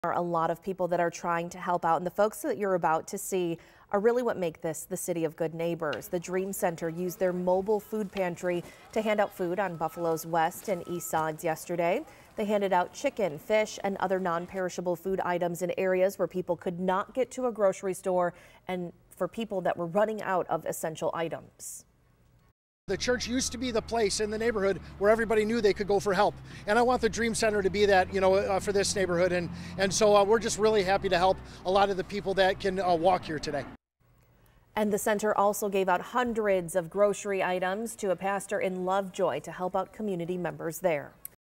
Buffalo Dream Center used mobile food bank to give away food
These folks we are writing about are what make this "The City of Good Neighbors."